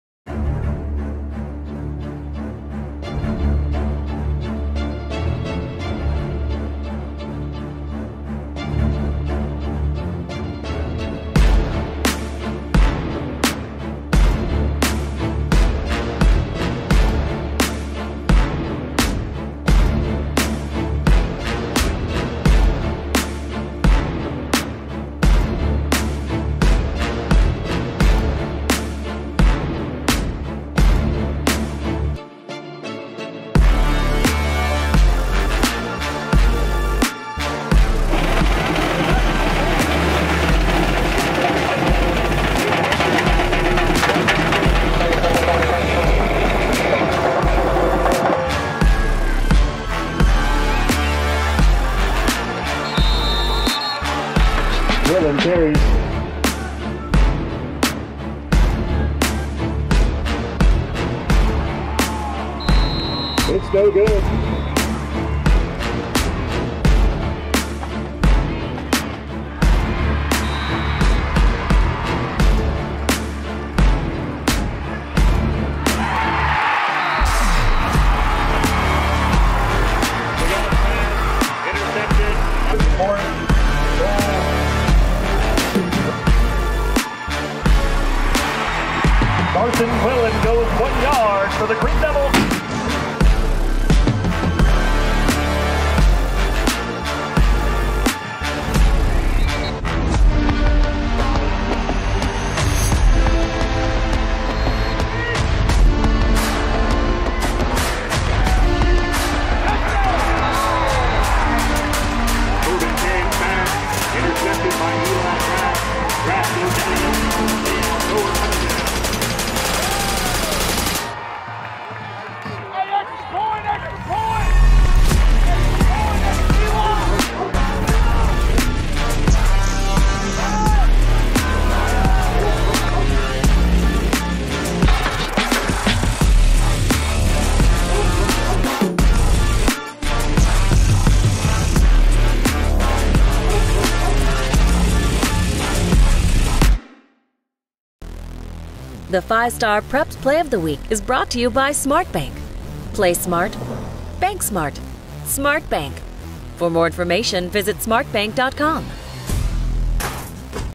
SmartBank FILM ROOM: Alcoa vs. Greeneville (2023)
SmartBank FILM ROOM
One of the most anticipated matchups of the 2023 season was cancelled just before the half due to lightning and storms. But that doesn't mean there weren't highlights! See some of the top plays and scenes between Alcoa and Greeneville from Week 4 of the 2023 season.